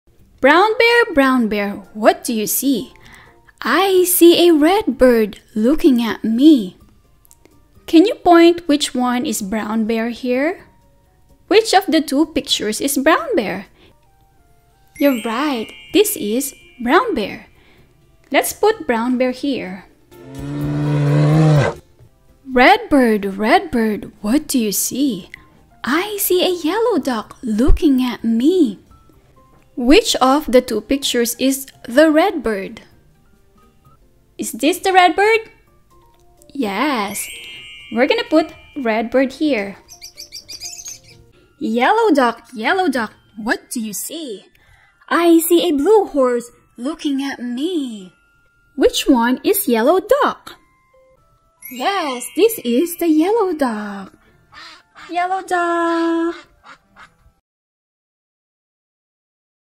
Brown Bear Brown Bear What Do You See Read Aloud
In today's circle time, Miss Kaye reads Brown Bear, Brown Bear, What Do You See? by Bill Martin Jr. and Eric Carle.This is the animated version of the book. Check out the full version in our channel.

Come and Learn with Miss Kaye!

Check our PRINTED BOOKS:
○ My Phonics Book: Letter Sounds